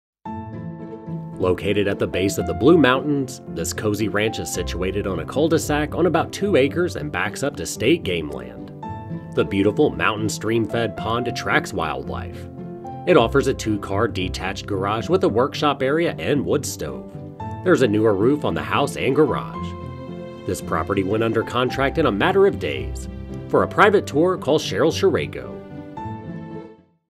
211 Camp Strauss Road Bethel, PA | ColdwellBankerHomes.com
211 Camp Strauss Road, Bethel, PA

Located at the base of the Blue Mountains, this cozy ranch is situated on a cul-de-sac on about two acres and backs up to state game land. The beautiful mountain stream fed pond attracts wildlife. Offers a two-car detached garage with workshop area and wood stove. There is a newer roof on the house